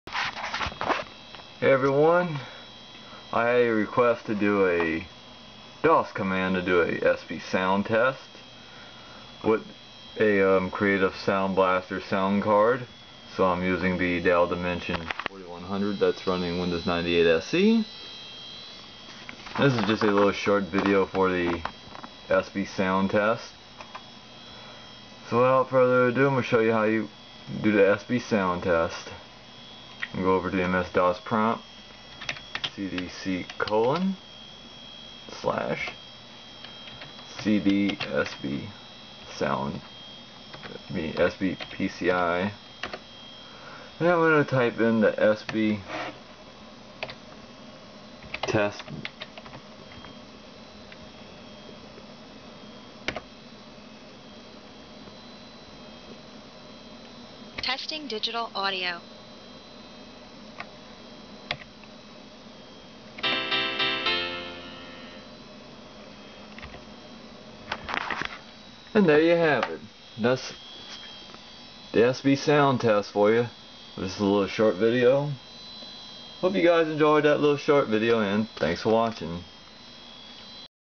Creative SoundBlaster Sound Card SBTEST
I had a request to do a video showing the DOS command for Creative SoundBlaster SBTEST with an actual SoundBlaster sound card, so this is very short video showing the SBTEST using the MS-DOS Prompt in Windows 98. I had my headphone plugged which it made hard for me to record with one hand holding the camera and headphones to make to this video. The SBTEST is supposed to test the DOS driver for any Creative SoundBlaster sound card. I was told the SBTEST won't work in VM-Ware or any emulator that I can run Windows.